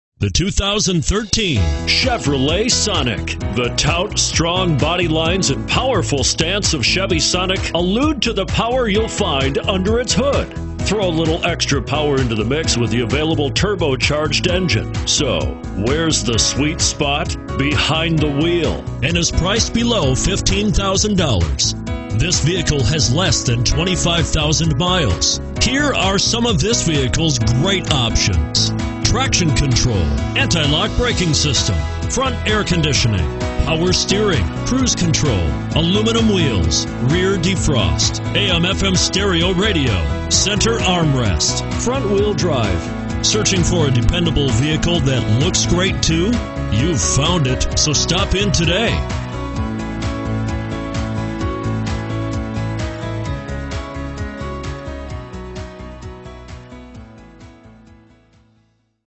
2013 Chevrolet Sonic Minneapolis St. Cloud & Monticello MN 25505B
2013 Chevrolet Sonic LT

Looking for the right car? Today could be your lucky day. With 20078 miles, this Victory Red, 2013 Chevrolet Sonic equipped with Automatic transmission could be yours. Request more information and set up a test drive right away.